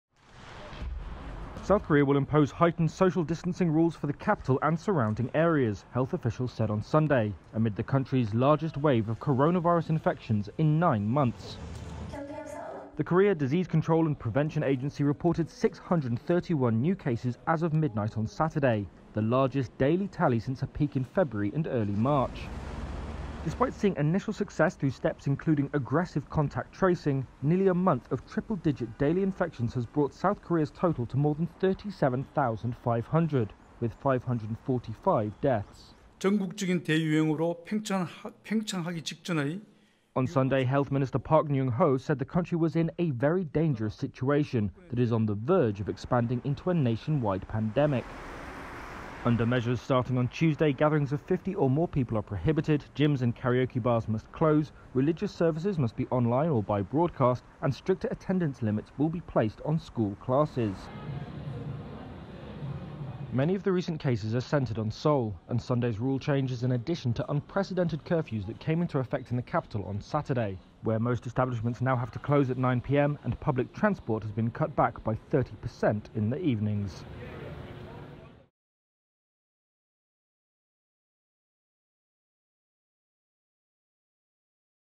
South Korea tightens restrictions amid Covid spike
A range of new measures are being implemented in South Korea as the country recorded its highest daily total of new coronavirus infections. Read the story

Video: Reuters